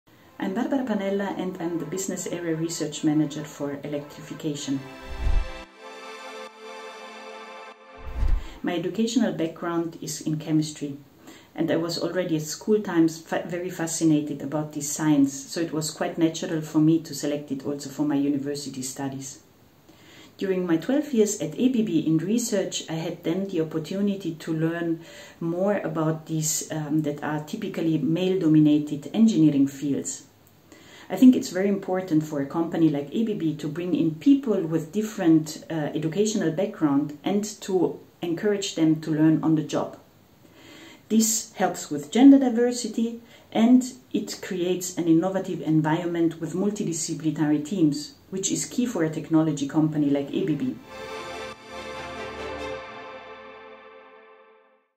ABB women in STEM - Barbara Panella
Barbara Panella, Research manager for ABB's Electrification Business Area, shares why it's important to recruit people from different educational backgrounds into technology companies such as ABB.